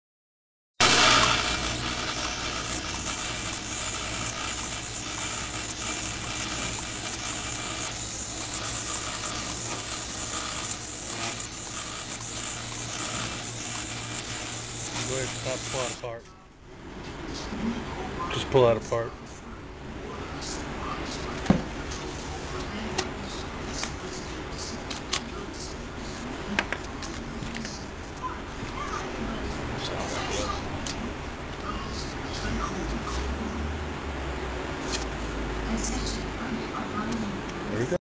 Die Cutting Solutions Inc Rotary Die Cutting .002" Copper with Adhesive Backing
Rotary die cutting .002" copper with adhesive backing for wireless antenna's